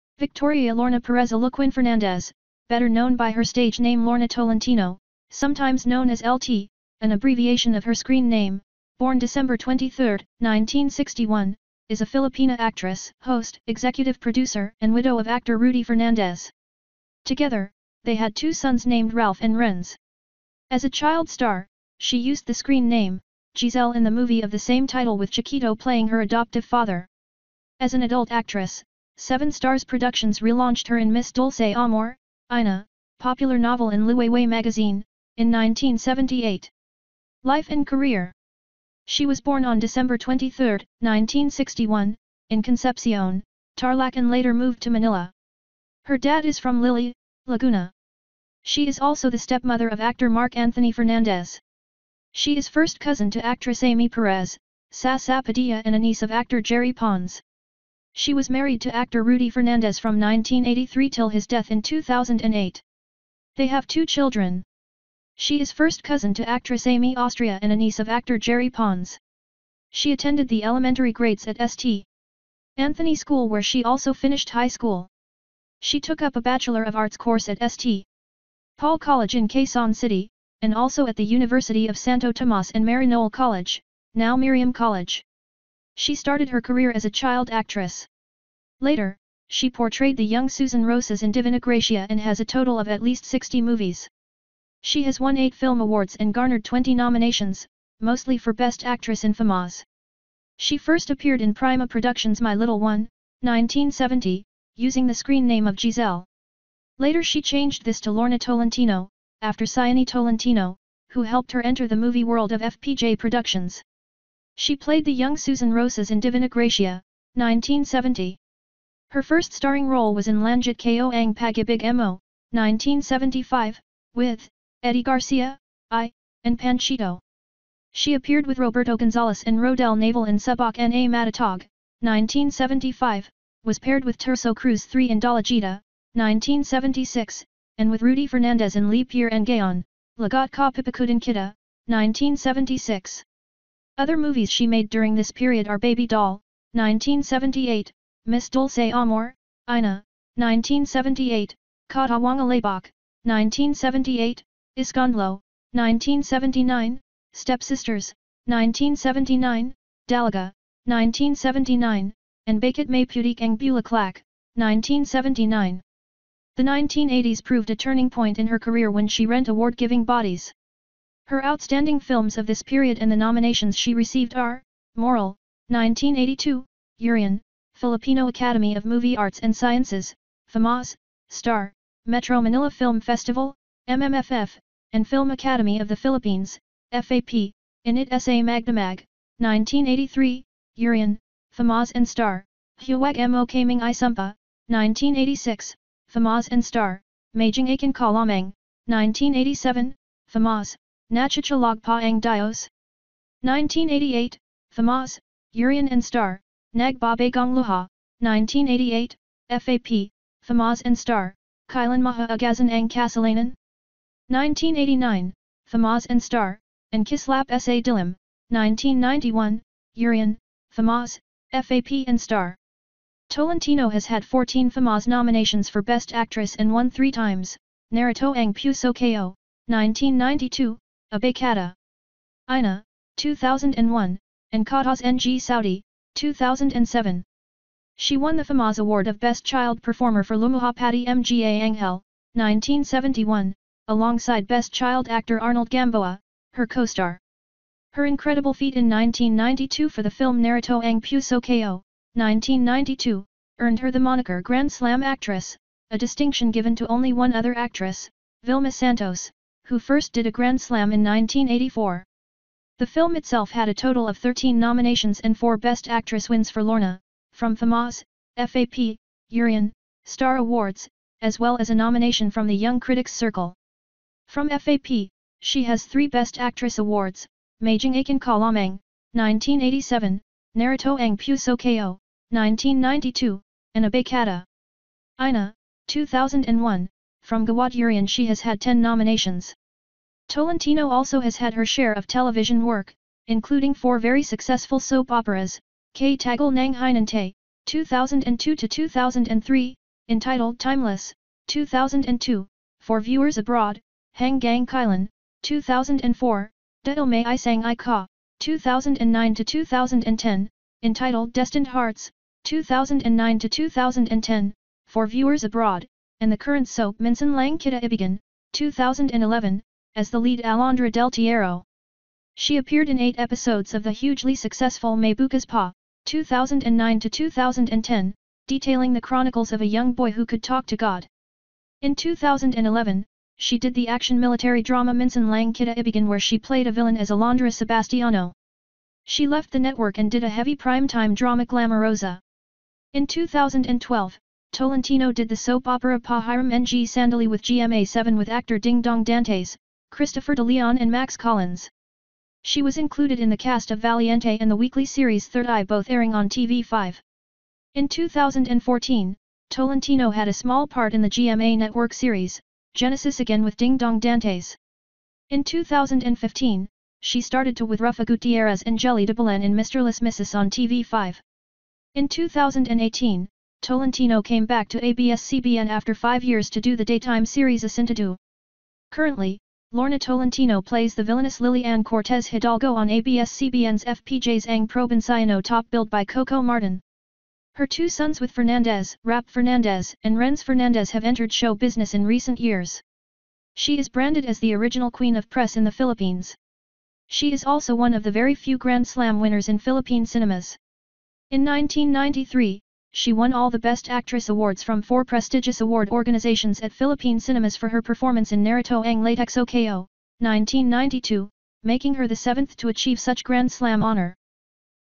Lorna Tolentino: Biography; Family; Career; Husband and More
*Thank you and wish you happy watching the video.:):):))

Victoria Lorna Perez Aluquin-Fernandez, better known by her stage name Lorna Tolentino, sometimes known as L.T., an abbreviation of her screen name (born December 23, 1961), is a Filipina actress, host, executive producer and widow of actor Rudy Fernandez. Together, they had two sons named Ralph and Renz. As a child star, she used the screen name, Giselle in the movie of the same title with Chiquito playing her adoptive father. As an adult actress, Seven Stars productions re-launched her in Miss Dulce Amor, Ina (popular novel in Liwayway Magazine) in 1978.
She was born on December 23, 1961, in Concepcion, Tarlac and later moved to Manila. Her dad is from Liliw, Laguna. She is also the stepmother of actor Mark Anthony Fernandez. She is first cousin to actress Amy Perez, Zsa Zsa Padilla and a niece of actor Jerry Pons. She was married to actor Rudy Fernandez from 1983 till his death in 2008. They have two children.
She is first cousin to actress Amy Austria and a niece of actor Jerry Pons.